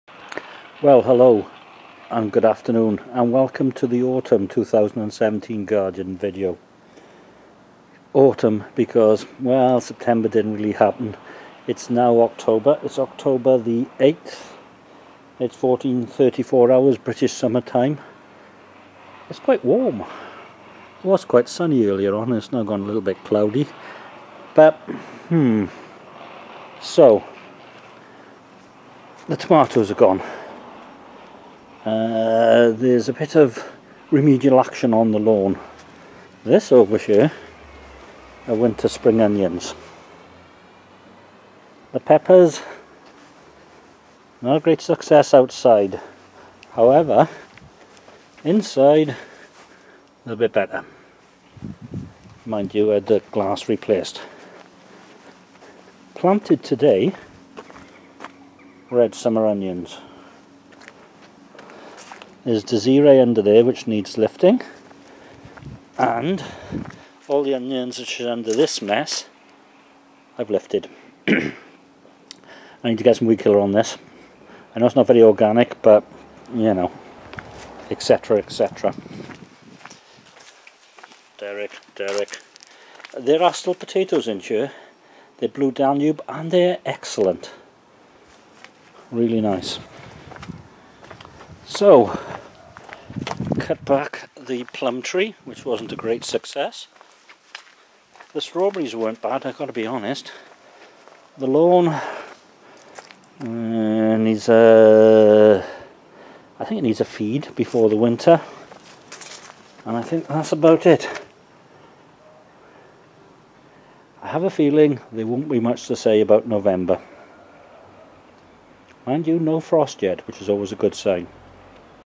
Garden - Autumn 2017
Due to various reasons, September didn't quite get recorded. Hence the Autumn (Fall) name. The tomatoes are finish and now composting. I've lifted the Onions and planted three containers of red summer onions. The lawn has been cut back and I've done some remedial grass seeding on it.

To come, I need to lift the Desiree, plant the rest of summer onions and treat the garden with some weed killer. Organic I'm not.